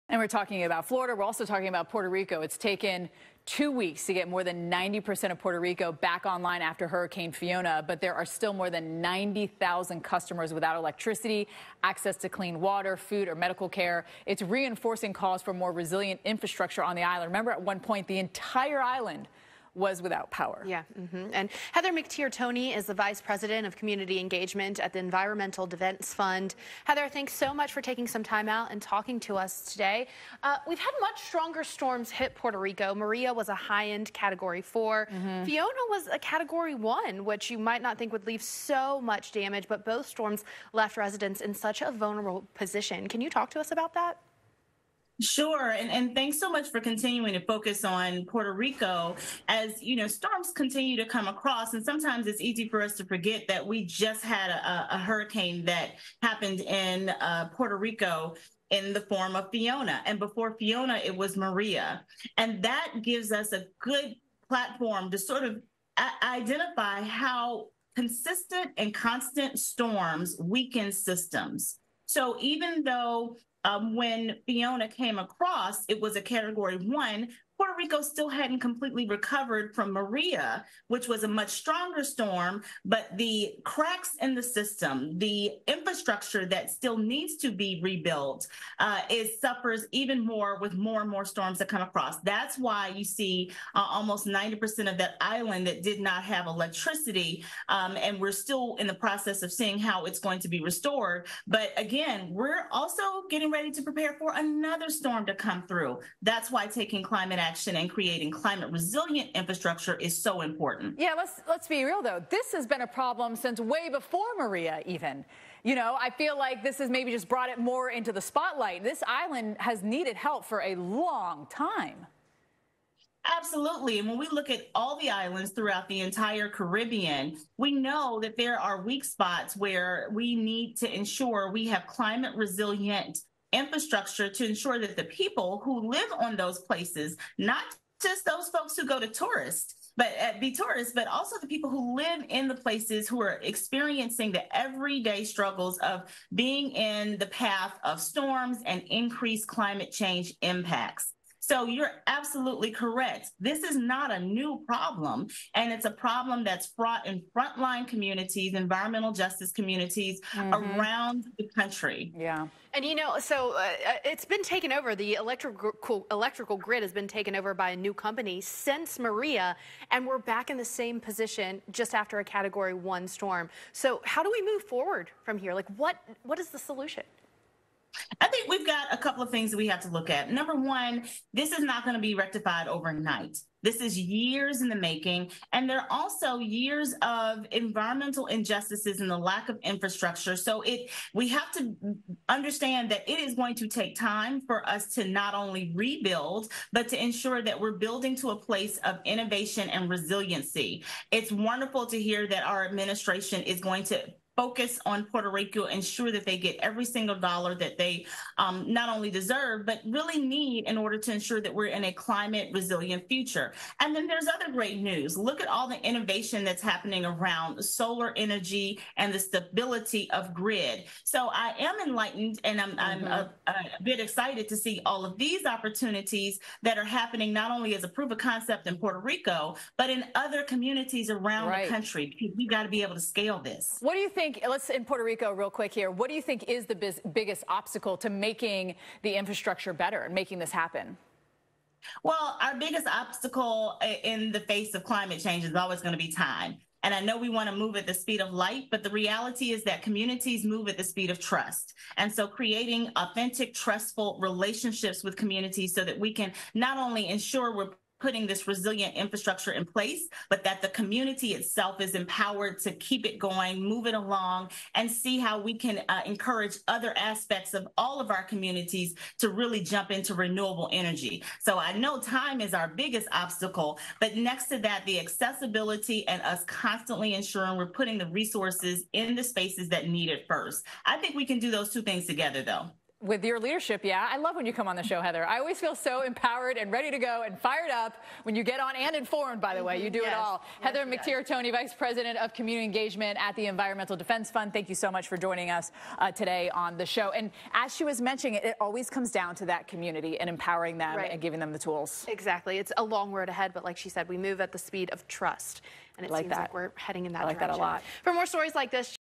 Rebuilding Stronger in Puerto Rico
EDF’s Heather McTeer Toney appeared on PattrnTV to discuss the path to rebuilding stronger in Puerto Rico in the wake of Hurricane Fiona.